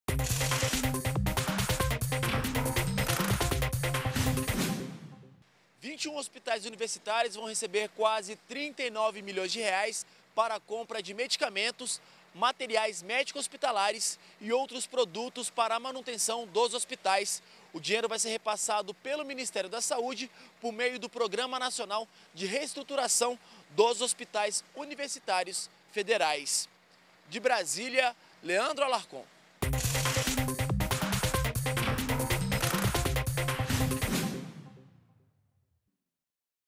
Ministério da Saúde repassa R$ 39 milhões para hospitais universitários
ÚLTIMAS - 05.12.14: O Ministério da Saúde vai repassar cerca de R$ 39 milhões por meio do programa Nacional de Reestruturação dos Hospitais Universitários Federais, para 21 instituições de todo o país. O repasse é para compra de medicamentos, manutenção dos hospitais e aquisição de matérias hospitalares.